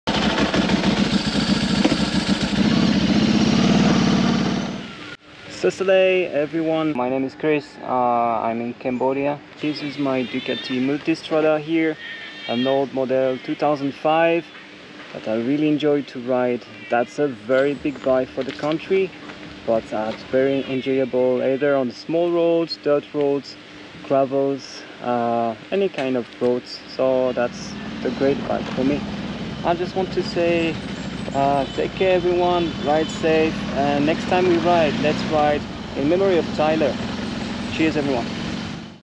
In Memory of Tyler. Message from Cambodia.
Here is a short video made in memory of Tyler.
People send videos from all around the world for a video memorial to remember Tyler Woelfersheim. Tyler passed away at the age of 18. A rider gone too young.